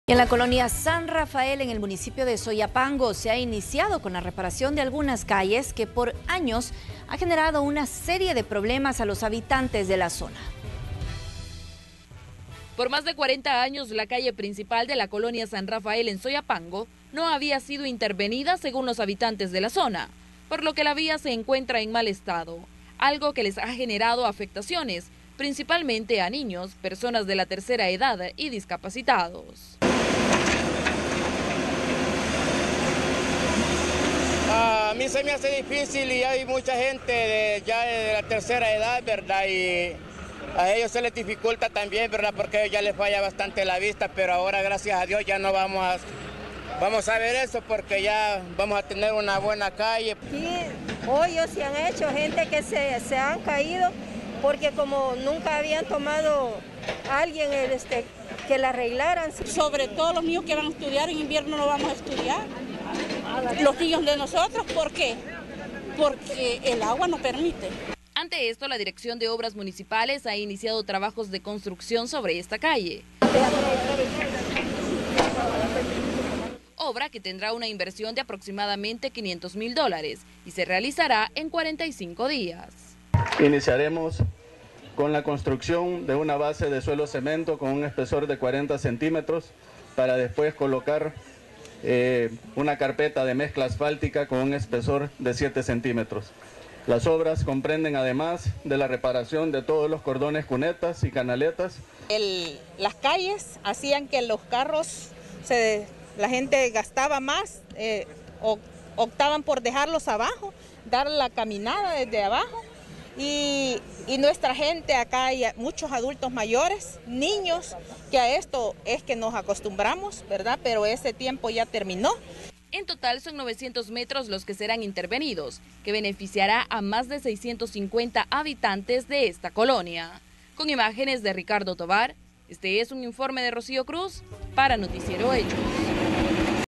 Municipio de San Rafael viene realizando mejoras de las calles en mal estado
Los habitantes de la zona venían siendo afectados por más de 40 años.
Las calles de la colonia de Soyapango eran un dolor de cabeza para los habitantes de la zona. Por más de 40 años estas vías serán arregladas para el beneficio de la comunidad. La obra tendrá una inversión aproximada de 500 mil dólares que se desarrollará en 45 días.